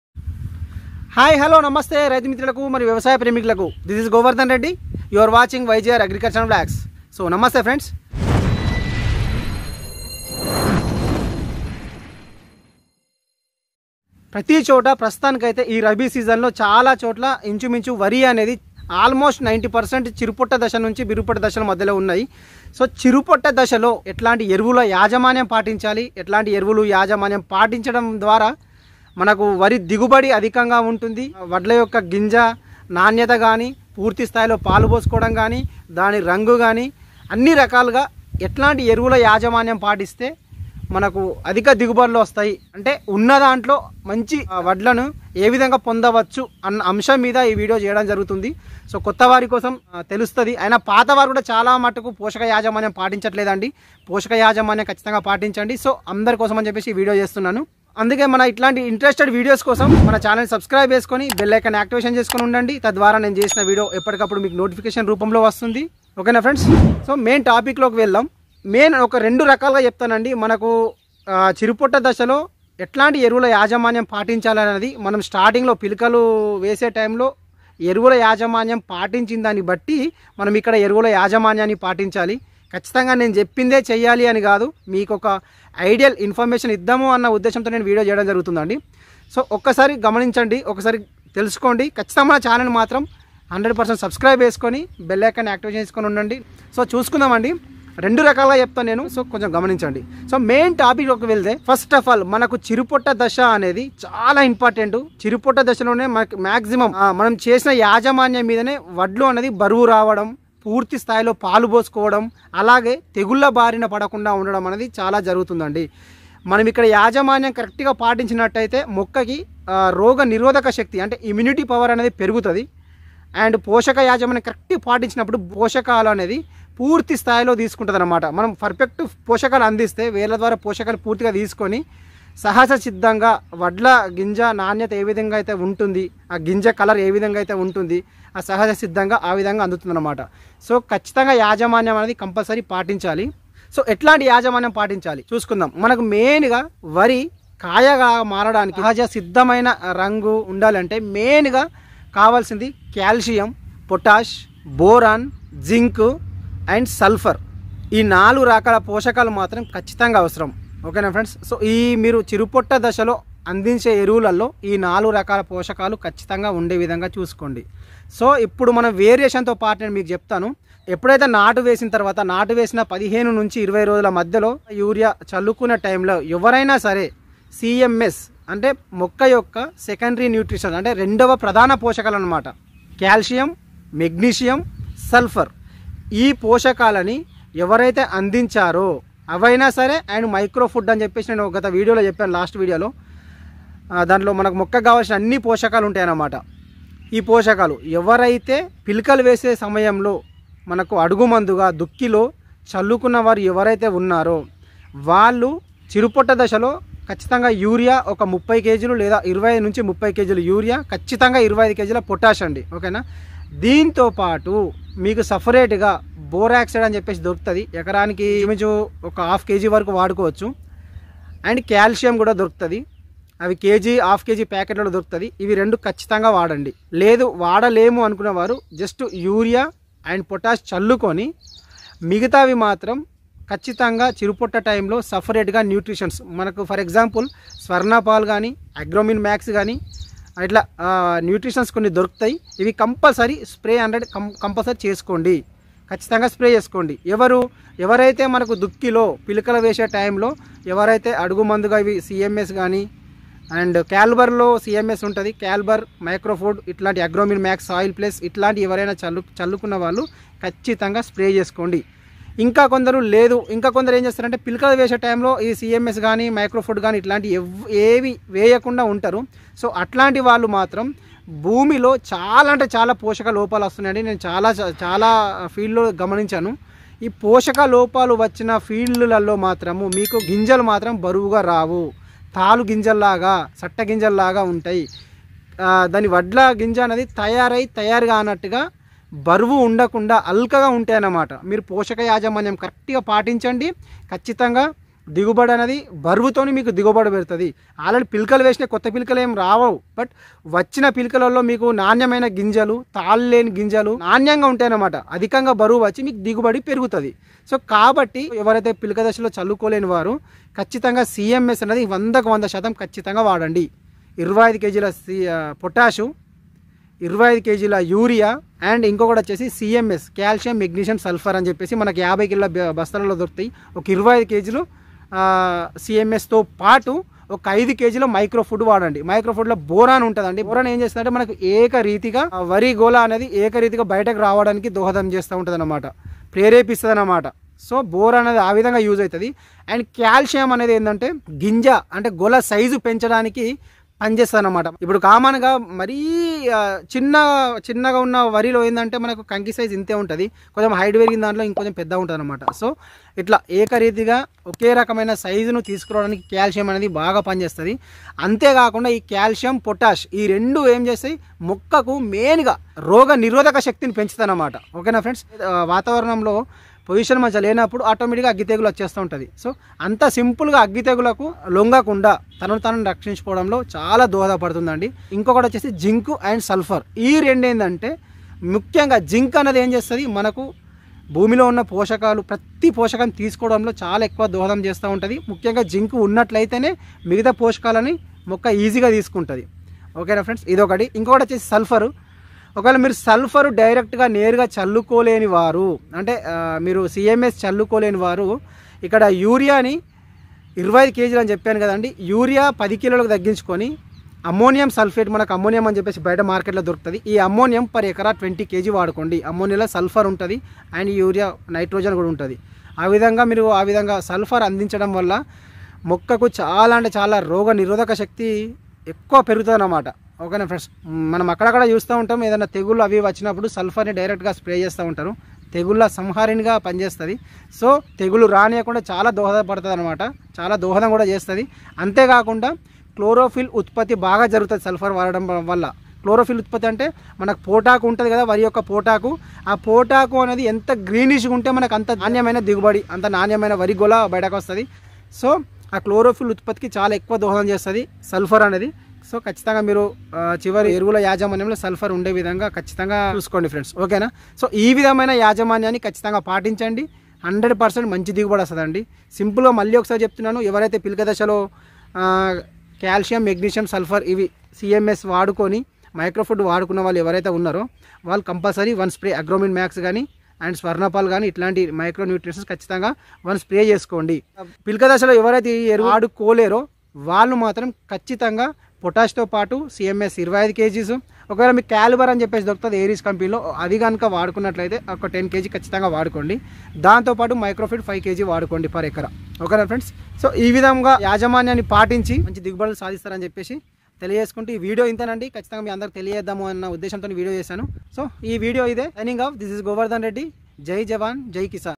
చిరుపొట్టదశలో మేలైన ఎరువుల యాజమాన్యం|chirupotta yeruvula yajamanyam|fertilizer management in paddy
రైతుమిత్రులకు నమస్కారం 🙏
   చిరుపొట్ట దశలో అతిప్రాముఖ్యమైన ఎరువుల యాజమాన్యం గురించి ఈ వీడియో లో తెలియజేసాను..కాబట్టి వీడియో పూర్తిగా చూసి విషయాన్ని గ్రహించగలరు..అలాగే మన ఛానల్ ని సుబ్క్రైబ్ చేసుకొని bell బటన్ ని ఆక్టివేట్ చేసుకొని ఉండగలరు.